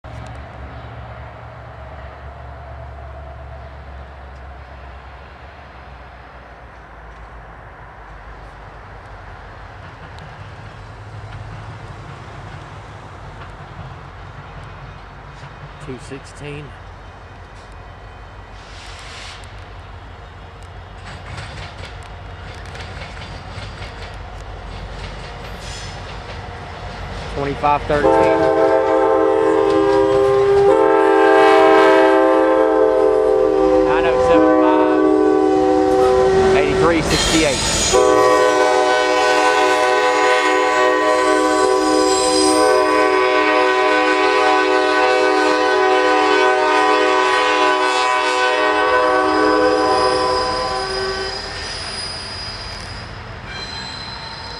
Norfolk Southern 216 NB Power w/ Cool Crew! Austell,Ga 12-01-2012© (16x9)
6:50PM in Austell,Ga is NS 216 Northbound power heading to Whitaker Yard to pick up their train.

Recorded by Gene and Tommy Hale on December 1, 2012